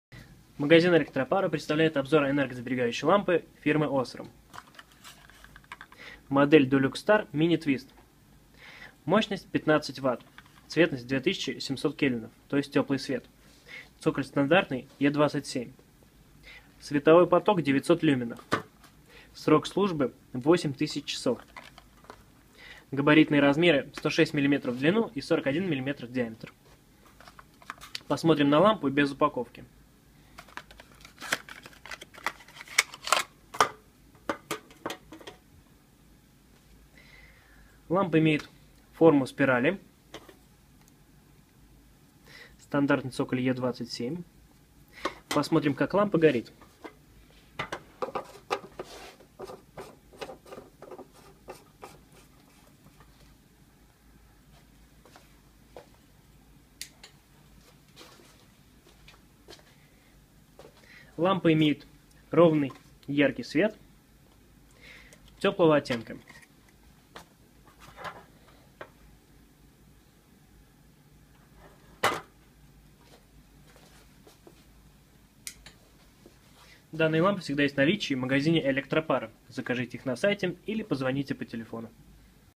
OSRAM DULUX MINI TWIST 15W/827 E27, обзор энергосберегающей лампы
OSRAM DULUX MINI TWIST 15W/827 E27, обзор энергосберегающей лампы от магазина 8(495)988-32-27 Москва, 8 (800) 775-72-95 бесплатно по России